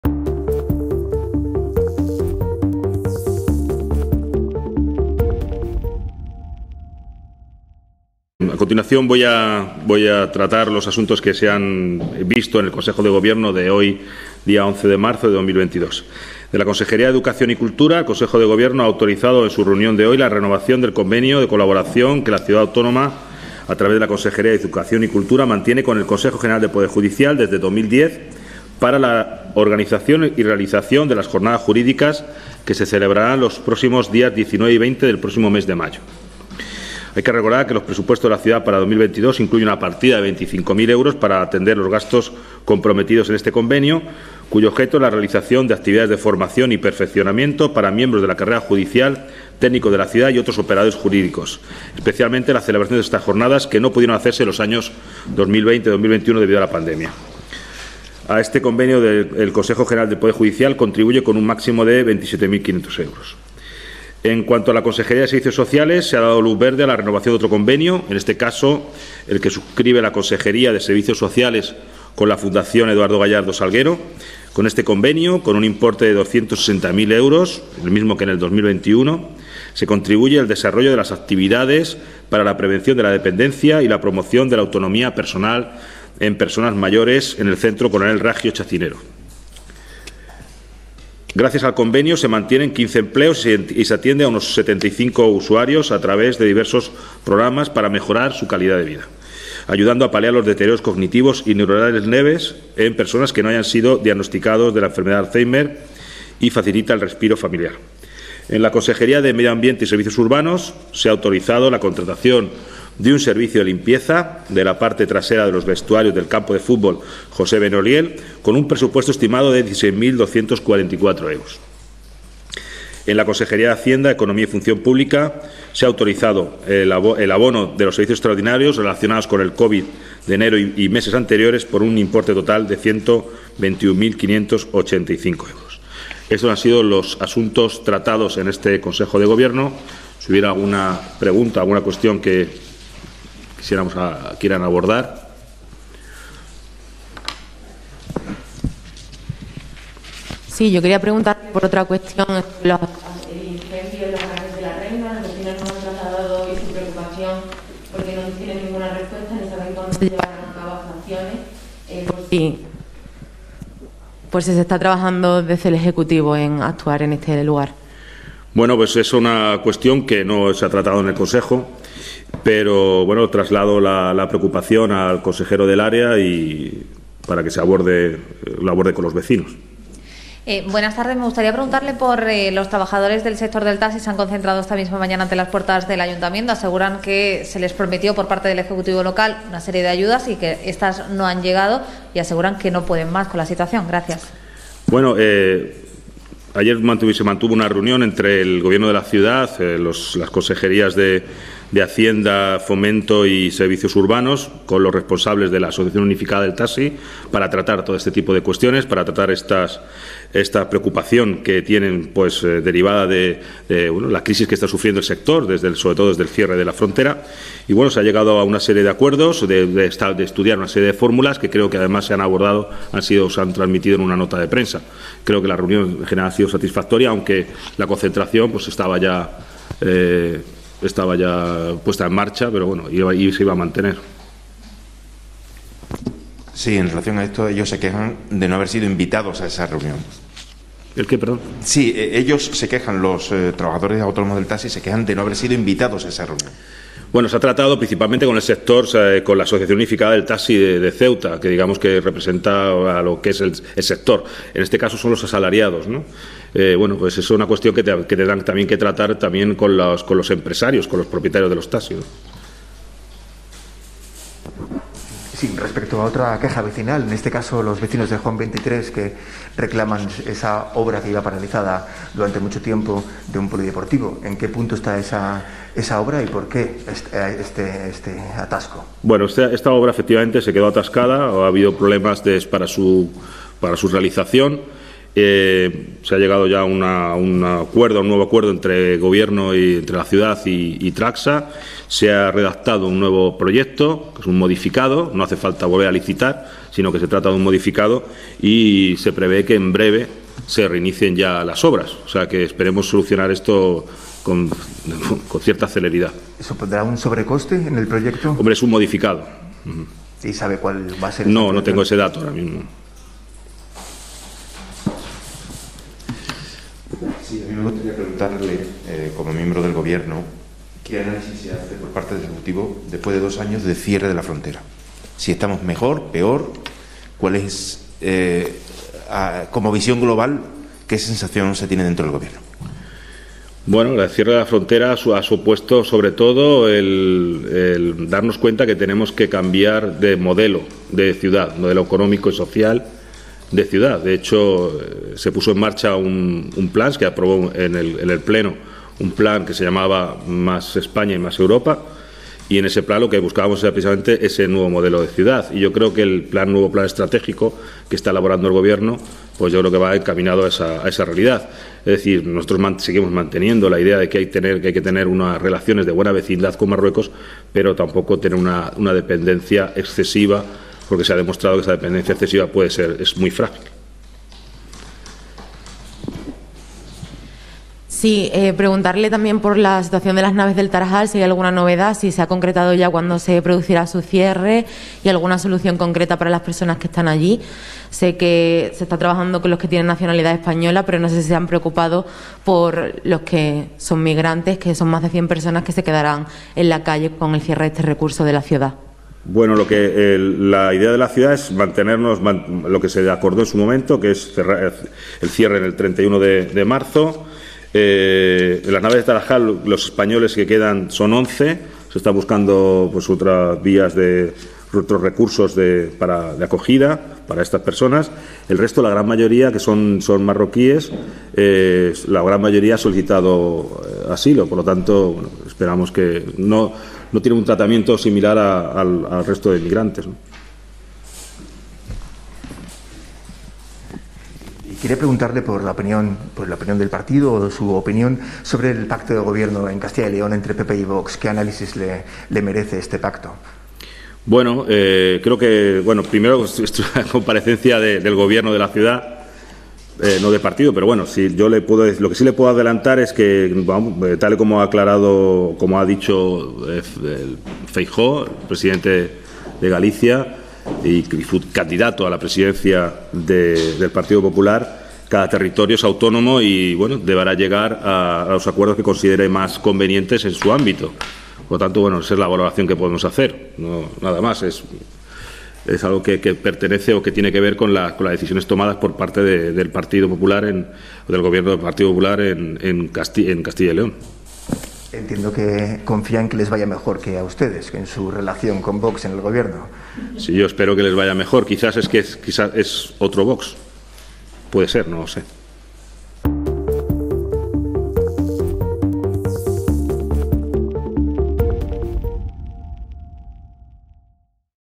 El Gobierno iniciará en breve la obra de la pista deportiva de Juan XXIII
Los trabajadores se han concentrado frente al Palacio de la Asamblea para hacer una fuerte pitada con la que exigir respuestas.

Imagen y edición: Cedidas